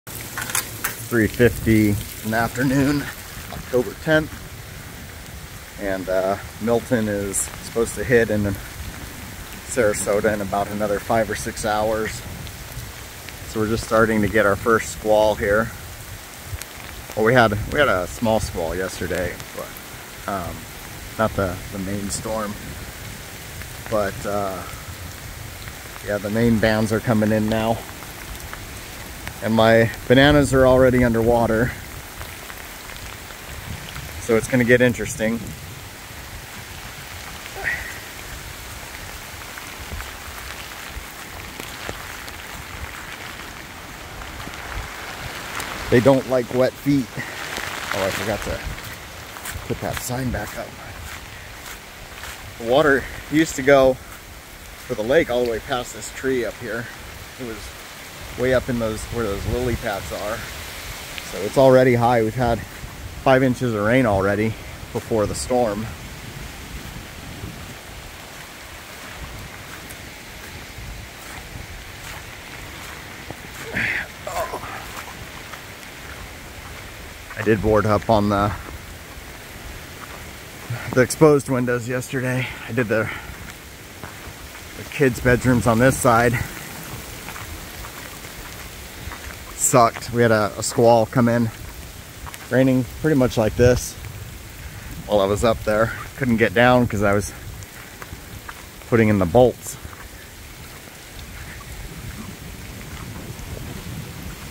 First bands from Hurricane Milton hitting Clermont.
Just downgraded to a Cat3 with 125mph winds. 152 miles from us right now.